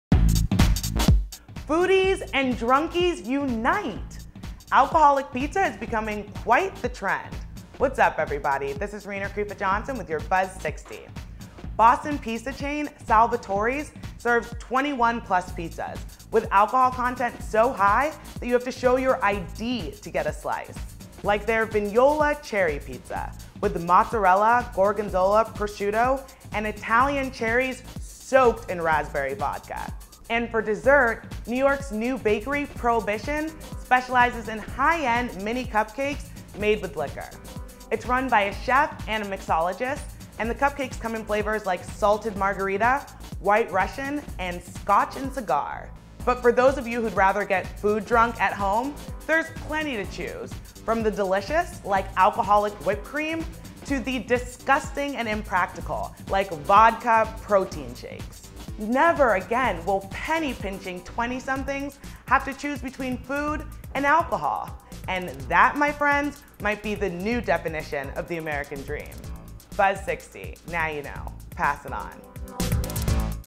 Foods that Will Get You Drunk: The Alcoholic Cuisine Revolution
Foodies and drunkies unite because alcoholic food is the newest culinary craze. Rena Karefa-Johnson introduces the trend, from liqour infused cupcakes to 21-plus vodka pizza pies.